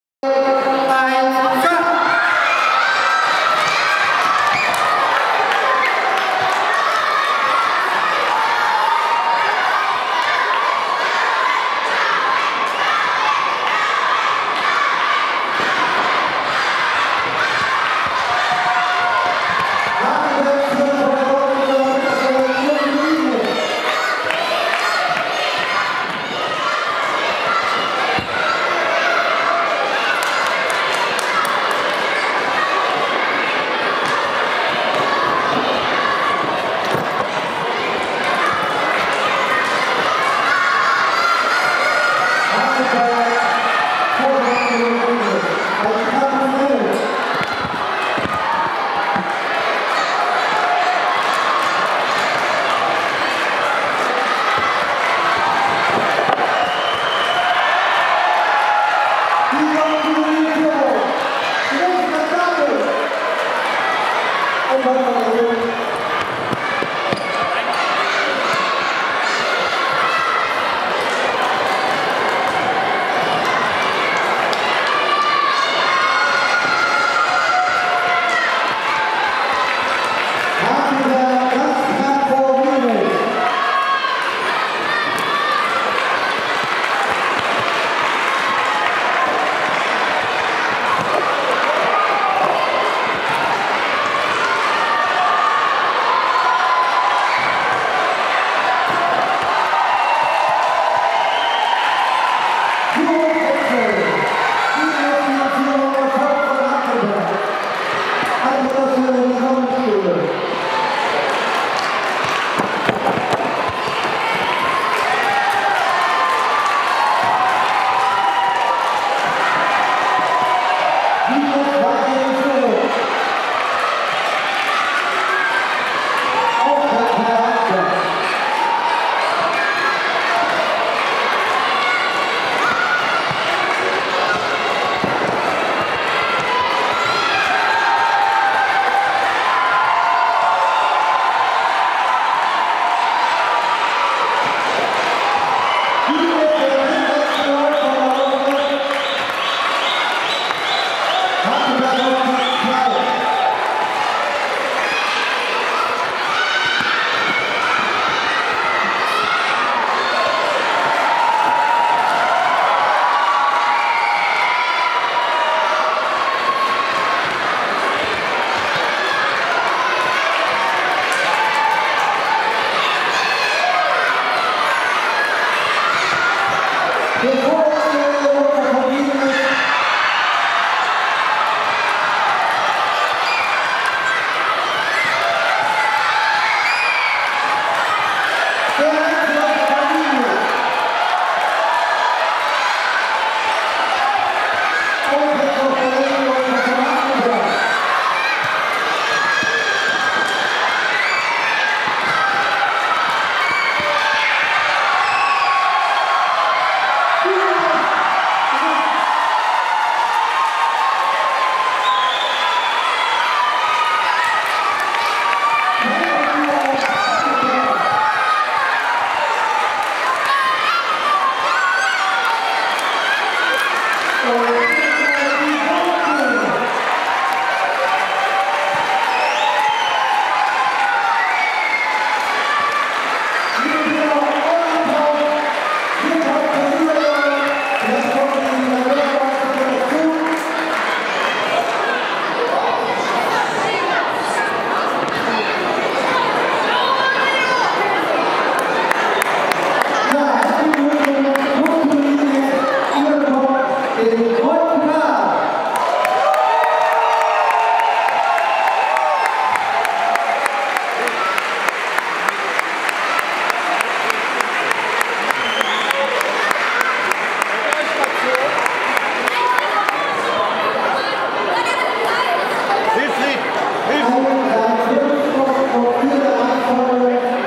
Grundschulstaffel Lauf (4k) 50. großer Sportabend Bergneustadt - Sieger GGS Wiedenest
Mehr - Grundschulstaffel Lauf (4k) 50. großer Sportabend Bergneustadt - Sieger GGS Wiedenest

Grundschulstaffel mit allen Grundschulen in Bergneustadt